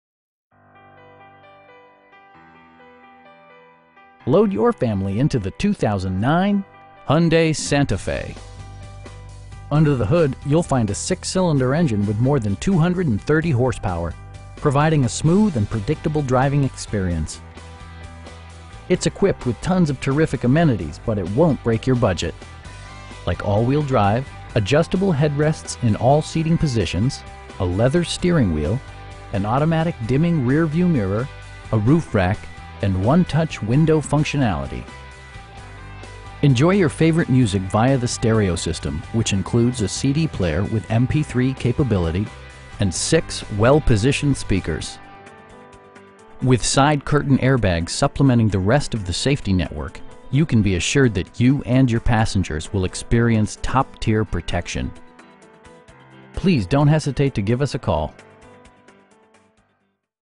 2009 Hyundai Santa Fe SE in Canandaigua, NY 14424
Canandaigua Chrysler Dodge Jeep

Load your family into the 2009 Hyundai Santa Fe. Under the hood you'll find a 6 cylinder engine with more than 230 horsepower, providing a smooth and predictable driving experience. It's equipped with tons of terrific amenities, but it won't break your budget. Like all wheel drive, adjustable headrests in all seating position a leather steering wheel, an automatic dimming rear-view mirror, front bucket seats, air conditioning, a roof rack, and 1-touch window functionality. With side curtain airbags supplementing the rest of the safety network, you can be assured that you and your passengers will experience top-tier protection. We have the vehicle you've been searching for at a price you can afford. Please don't hesitate to give us a call.